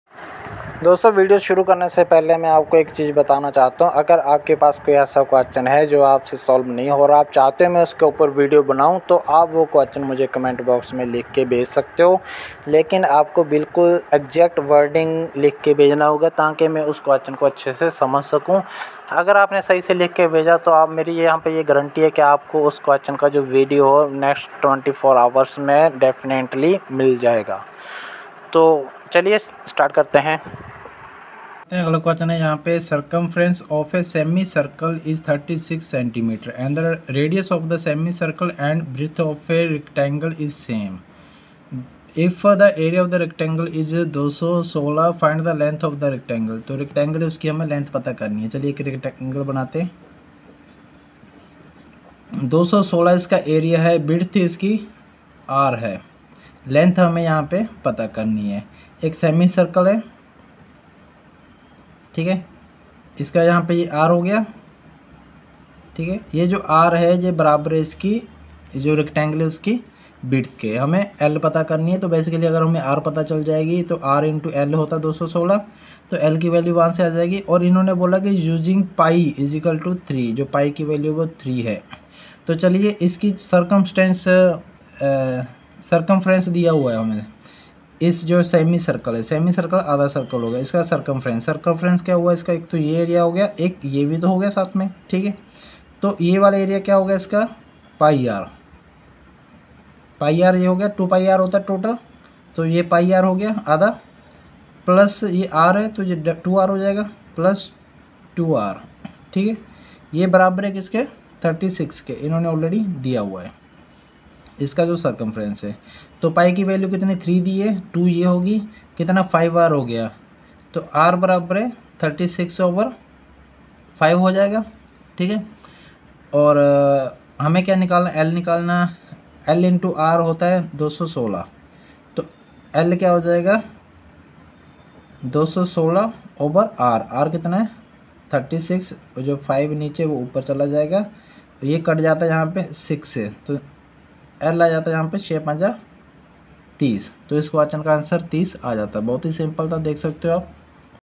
CIRCUMFERENCE OF THE SEMICIRCLE IS 36 CM AND THE RADIUS OF SEMICIRCLE AND BREADTH OF A RECTANGLE IS
CIRCUMFERENCE OF THE SEMICIRCLE IS 36 CM AND THE RADIUS OF SEMICIRCLE AND BREADTH OF A RECTANGLE IS, SAME. IF THE AREA OF RECTANGLE IS 216CM2 FIND THE LENGTH OF Tho Rectangle. [USING П=3]

Follow us on following